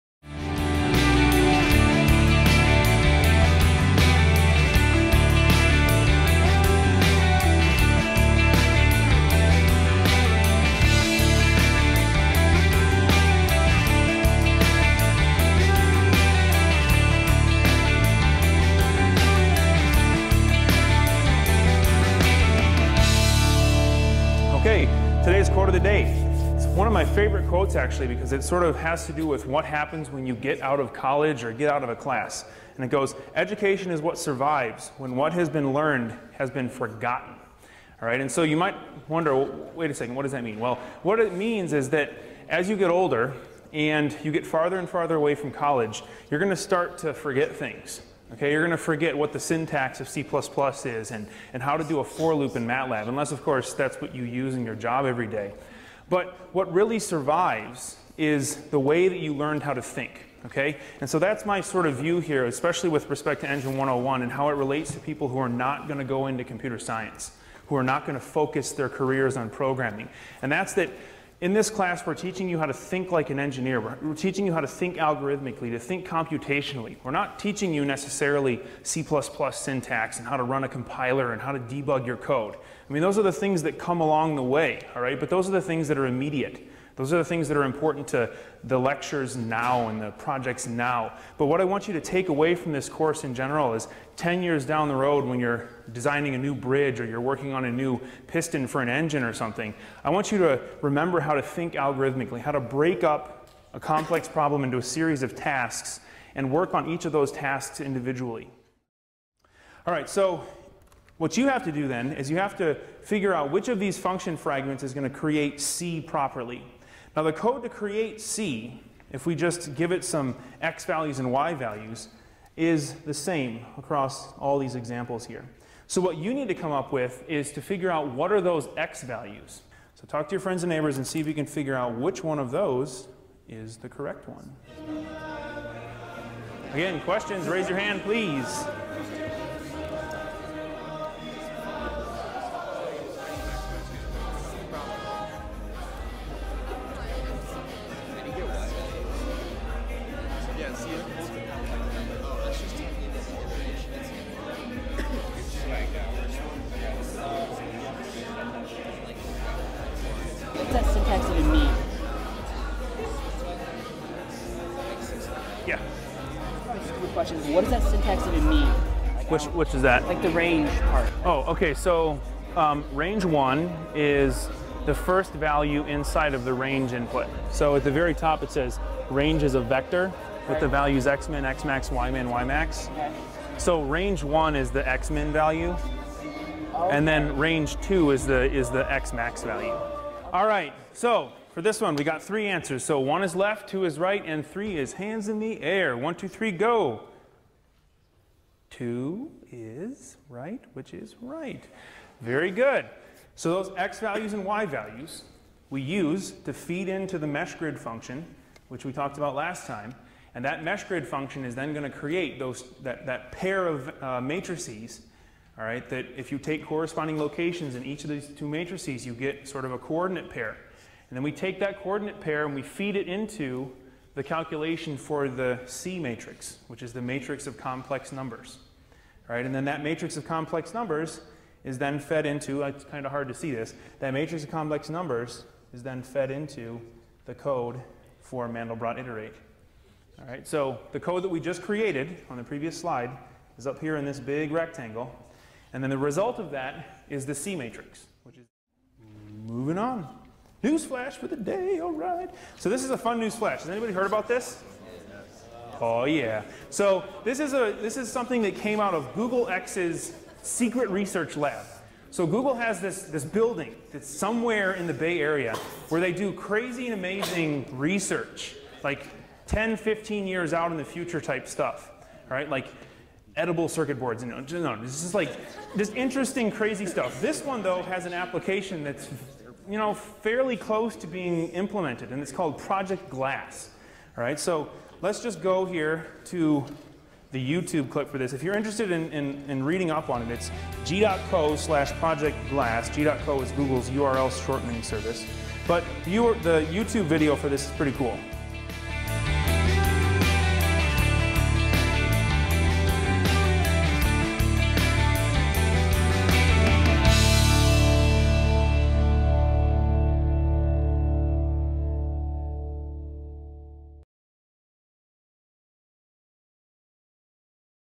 Engaging Students in Active Learning
This video highlights Jeff Ringenberg, Lecturer IV in Electrical Engineering emphasizing 'big picture' ideas, using active learning, and connecting concepts to the real world in order to engage students in a large lecture course.